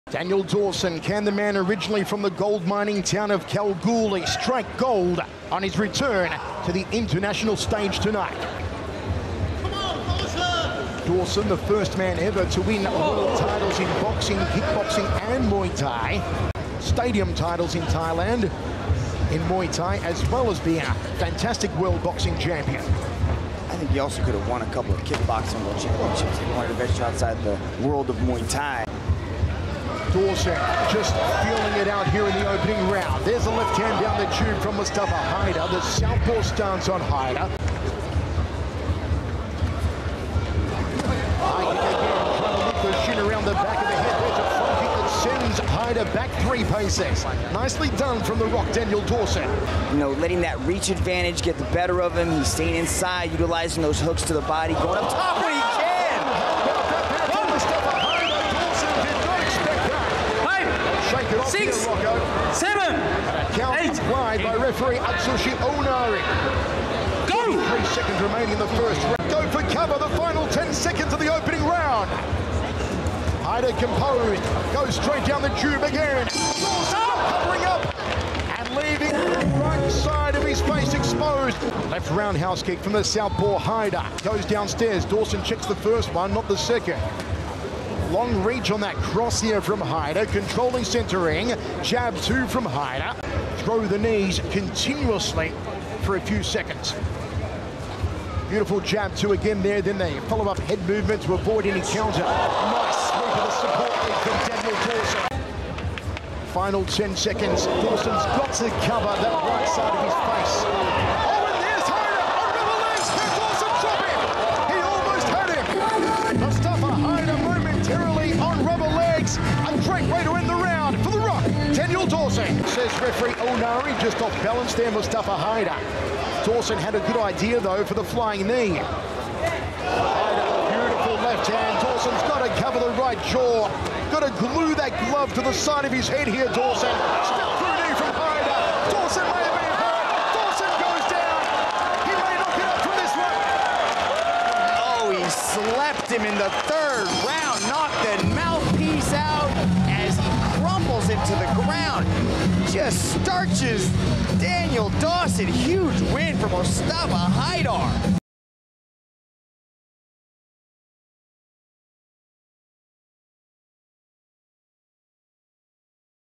Laga Sorotan | Haida VS Dawson | Highlight ONE
Inilah sorotan laga ONE Super Series divisi lightweight kickboxing antara Mustapha Haida dan Andy Dawson!

Jangan lewatkan aksi Haida di laga utama ONE: FISTS OF FURY III, dimana ia akan berjuang dalam perebutan gelar Juara Dunia ONE Lightweight Kickboxing melawan sang penguasa divisi, Regian Eersel, pada Jumat, 19 Maret ini.